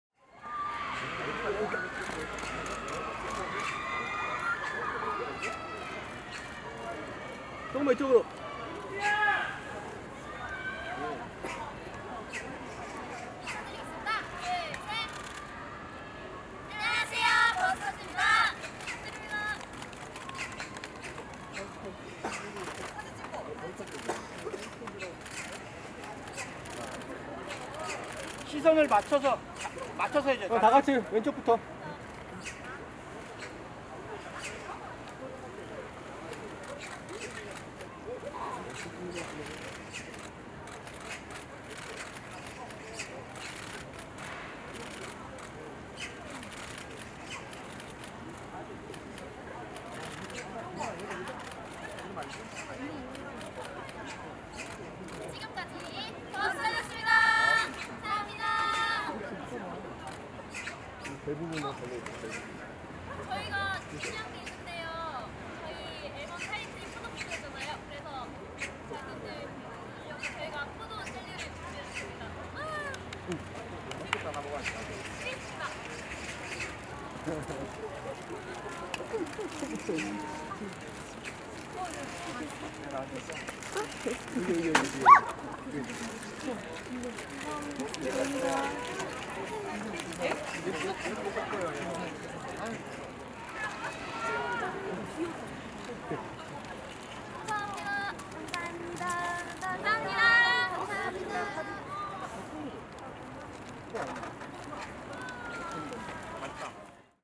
[Nine Stars] 버스터즈 Busters 포도포도해! 뮤직뱅크 출근길
©The JH Media 6월 22일 오전 KBS 뮤직뱅크에 버스터즈가 리허설 녹화에 참여하기 위해 출근하고 있다.